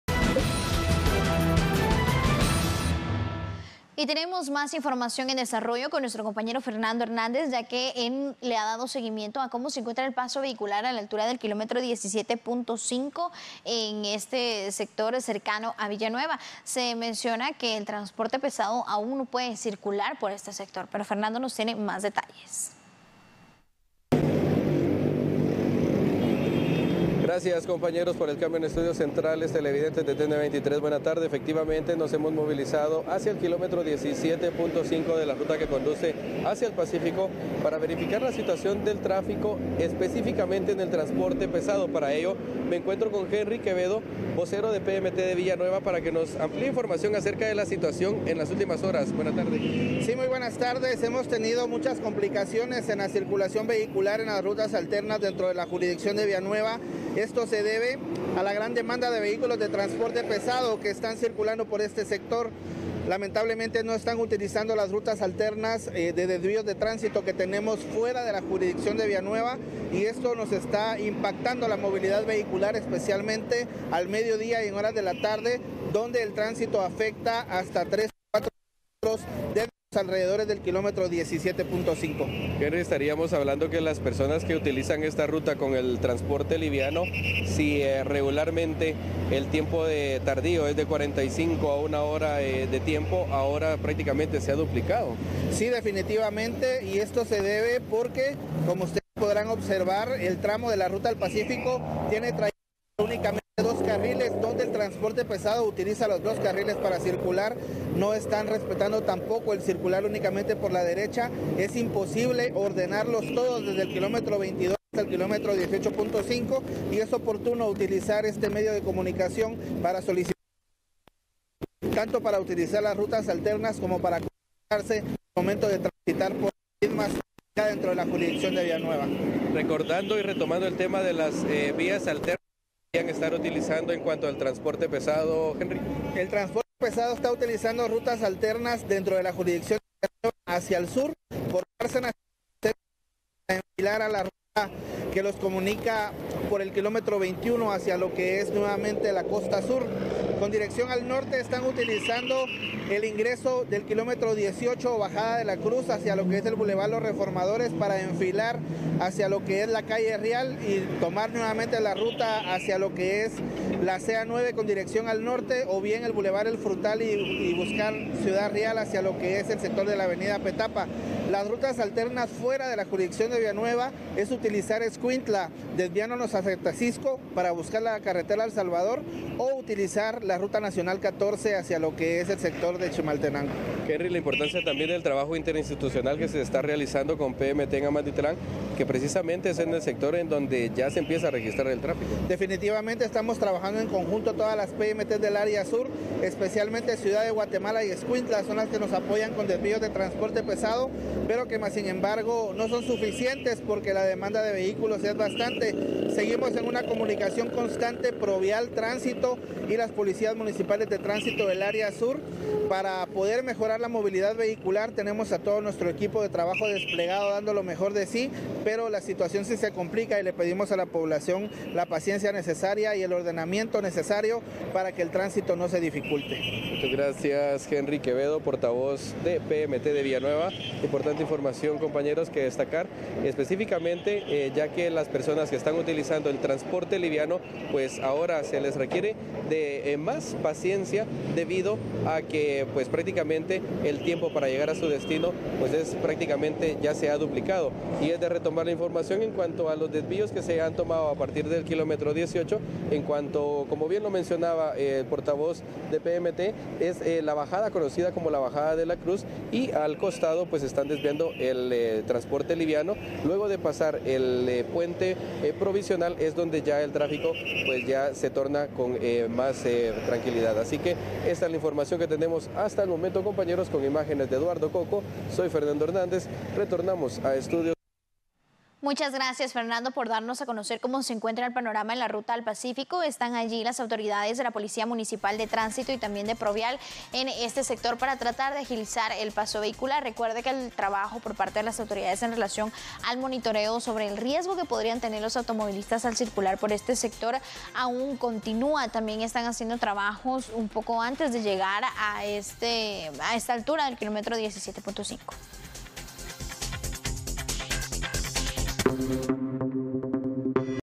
Complicaciones de circulación en ruta al Pacífico
Al respecto informa el periodista, Fernando Hernández.